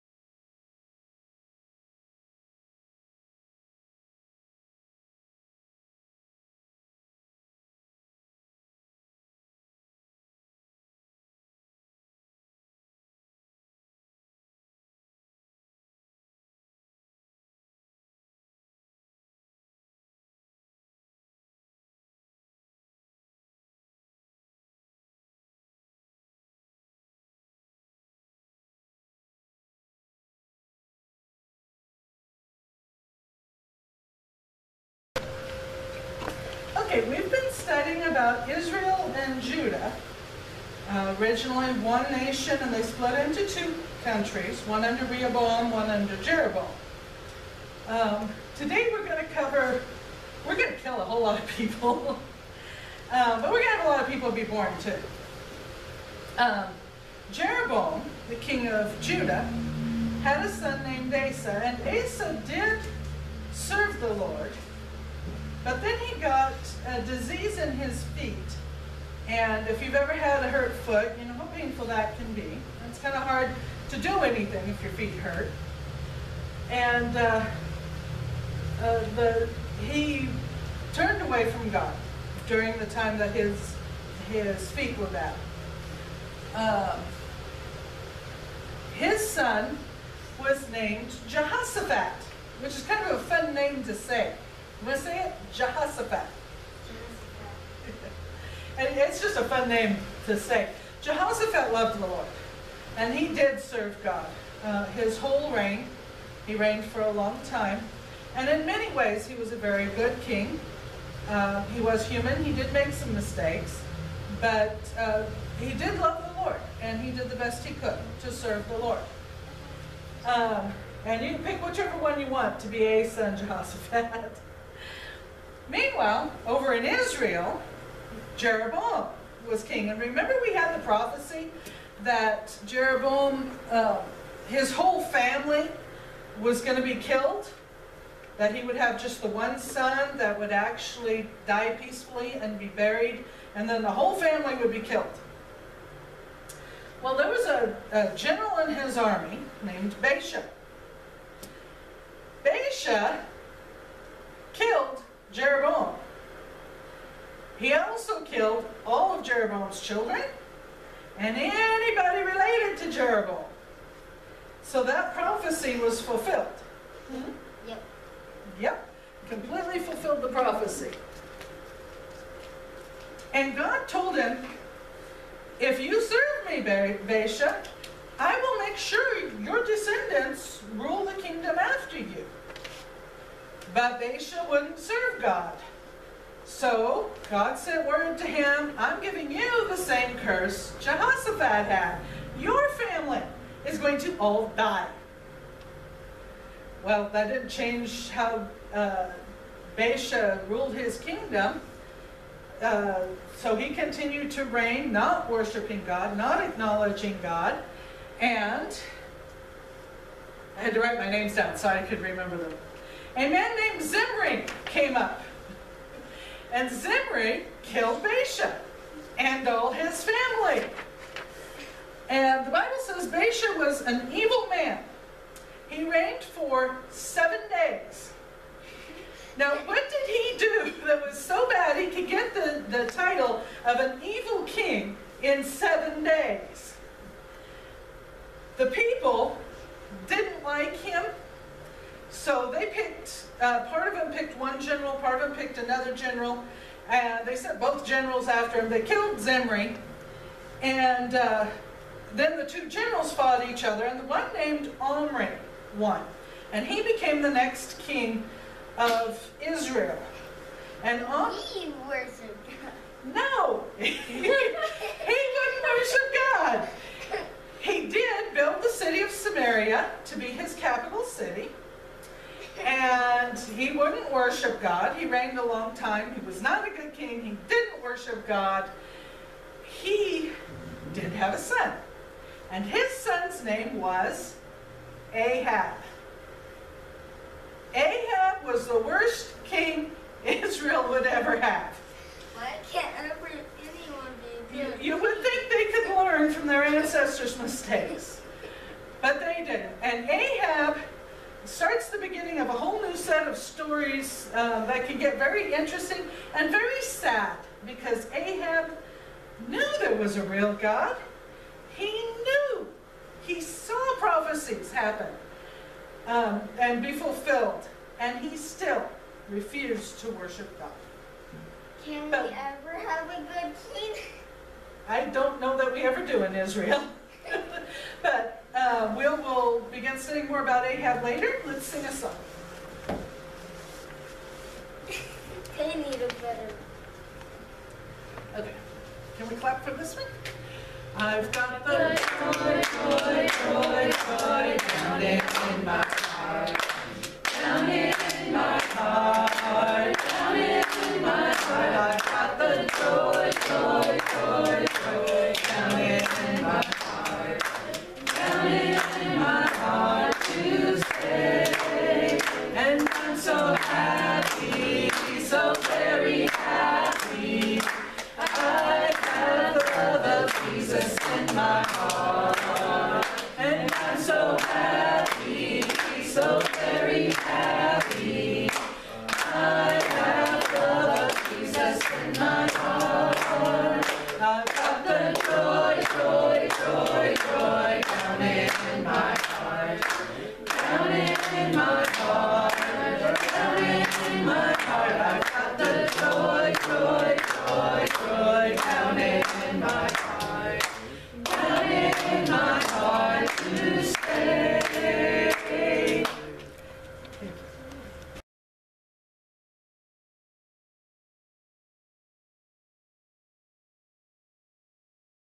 Bread of Life Christian Fellowship ~ The Children's Lesson ~ Kings, Kings Everywhere
by Sis. Betty Tracy